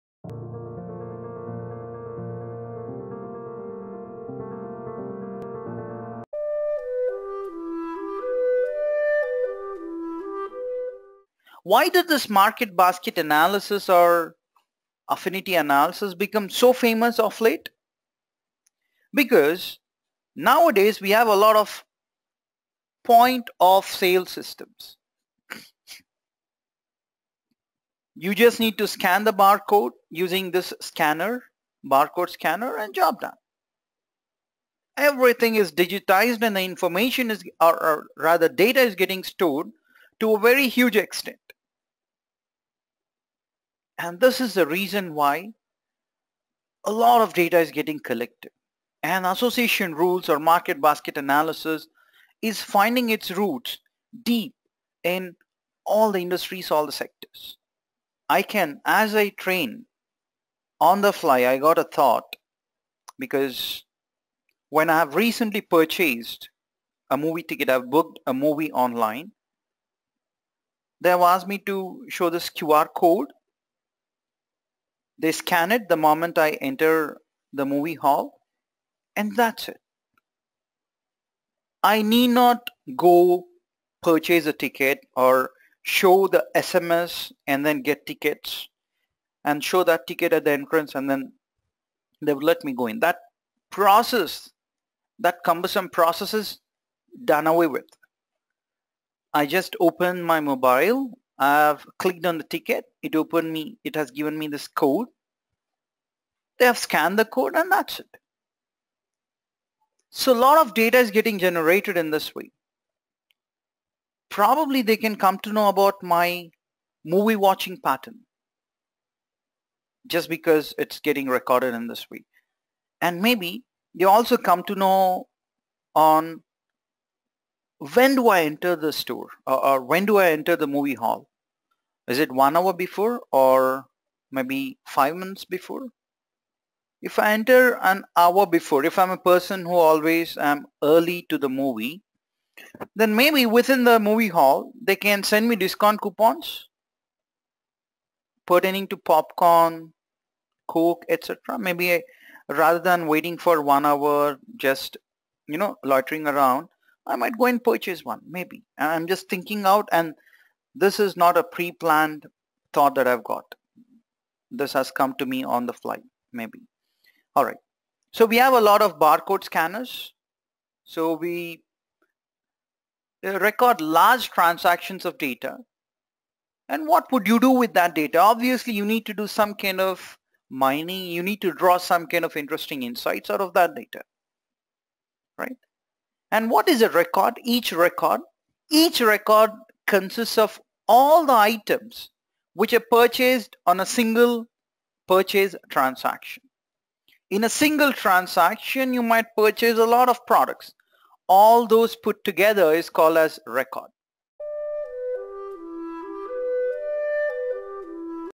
Data Mining Tutorial : What Is Market Basket Analysis ? | "Data Science" Tutorial [2018] - ExcelR
Market Basket Analysis is one of the key techniques used by large retailers to uncover associations between items. It works by looking for combinations of items that occur together frequently in transactions.

Toll Free (IND) : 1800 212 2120